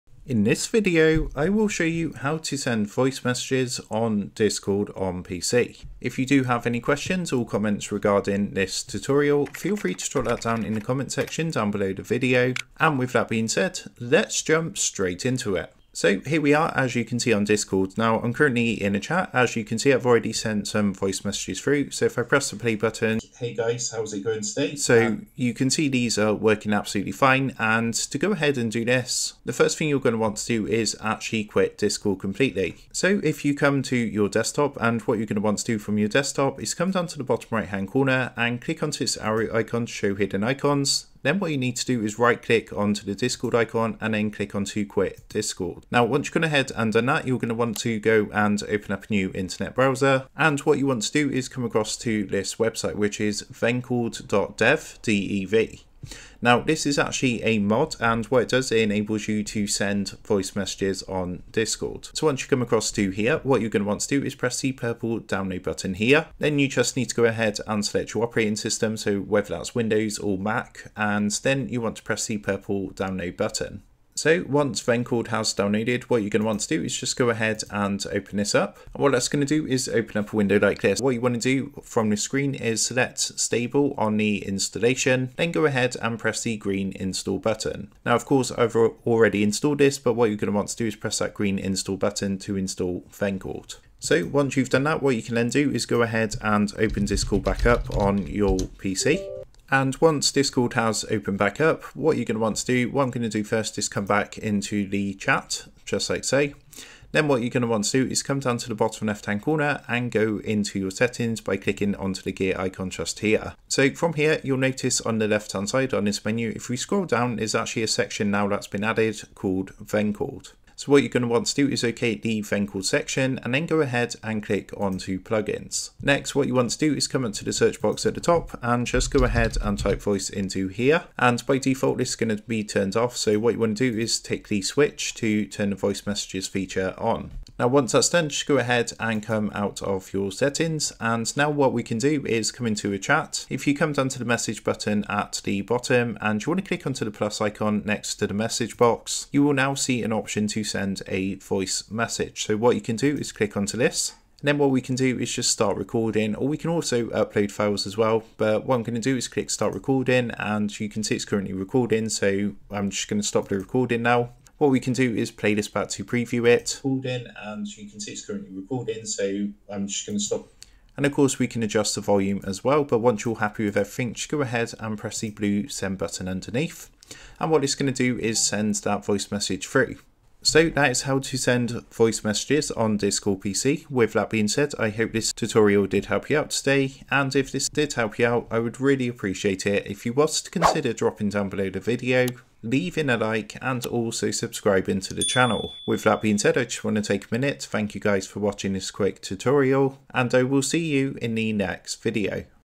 How To Send Voice Messages On Discord Pc
In this tutorial, we will show you how to send voice messages on Discord using your PC. Sending voice messages can add a personal touch to your communication on Discord and make it more engaging. To get started, simply follow the steps outlined in this video guide. We will walk you through the process of recording your voice message, selecting the recipient, and sending it seamlessly. By the end of this tutorial, you'll be able to confidently use voice messages on Discord to enhance your interactions with friends and fellow Discord users. Elevate your Discord experience with this easy-to-follow tutorial!

 ✨Recommended VPN: ✨

Thanks!